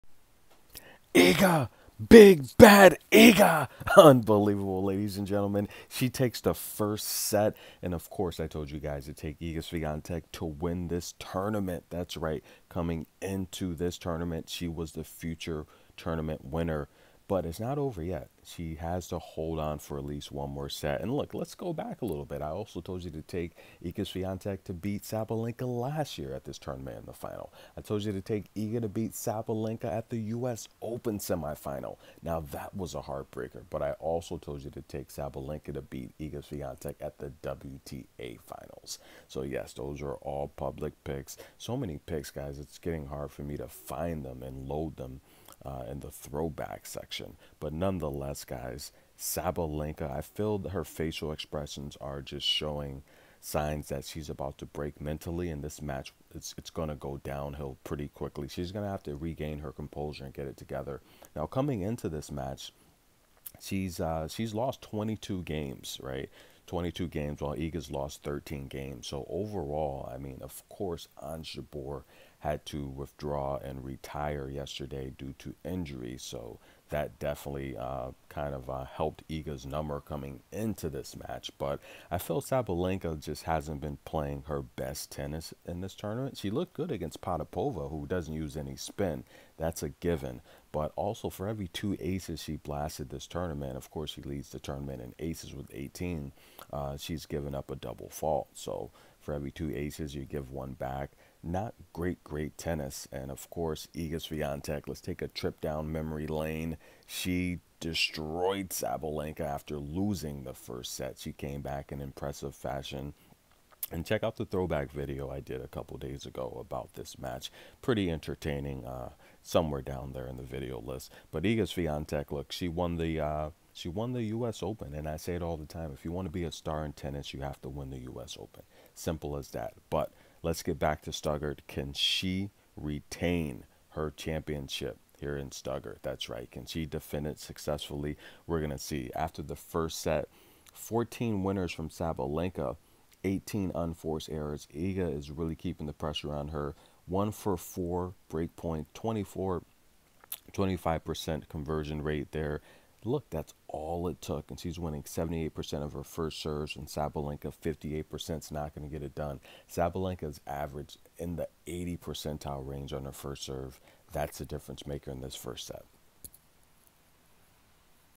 Iga Swiatek wins first set Vs Aryna Sabalenka Live Tennis WTA Stuttgart
Iga is clutch